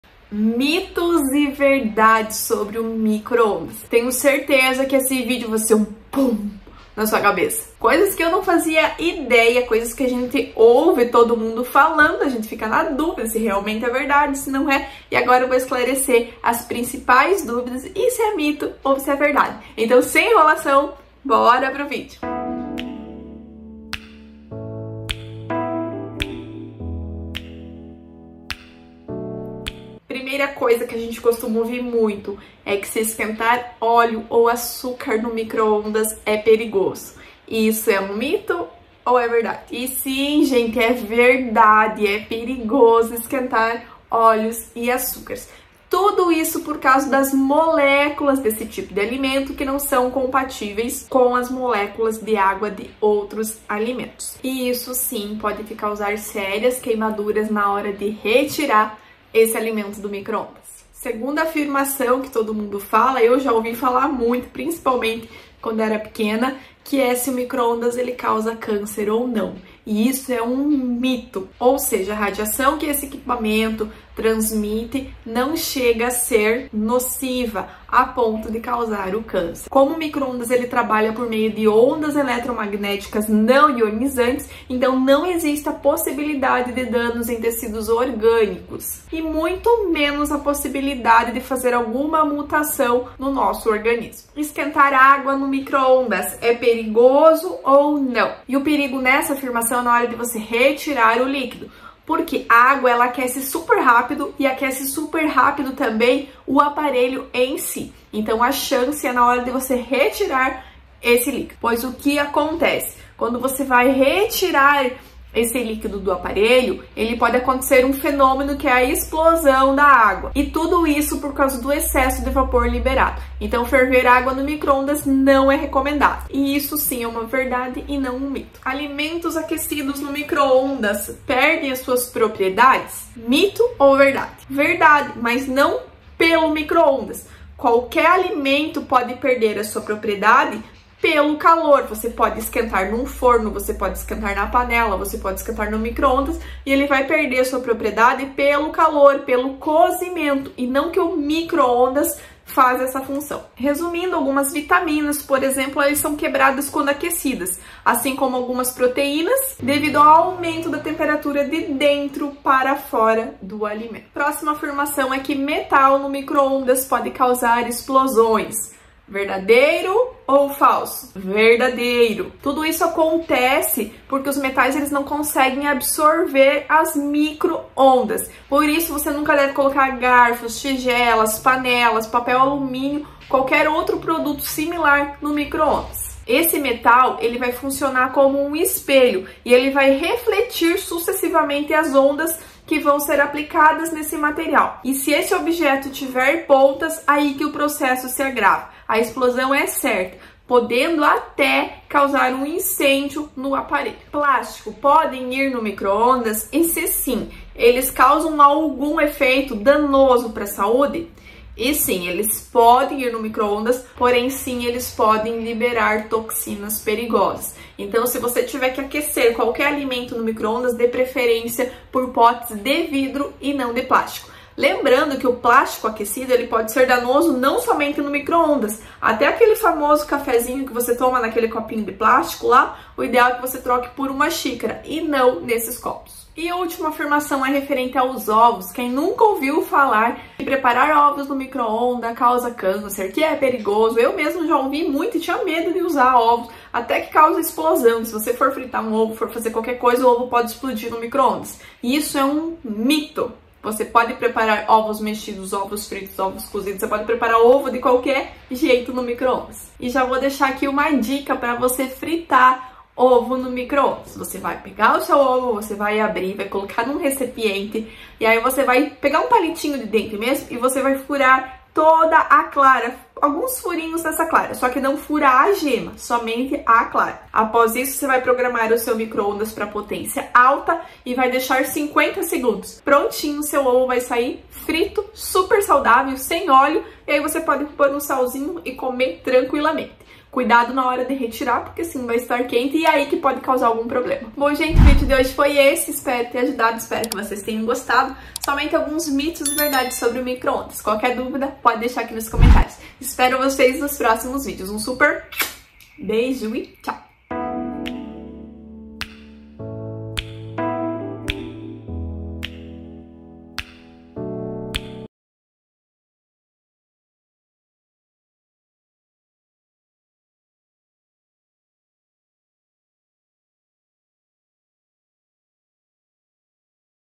MITOS E VERDADES SOBRE O MICRO-ONDAS
O micro-ondas é cercado de mitos, e nesse vídeo vou esclarecer os mais comuns. Será que o micro-ondas é cancerígeno? Podemos fritar ovos no micro-ondas? E esquentar água? E óleo?
Essa e mais algumas dúvidas bastantes comuns.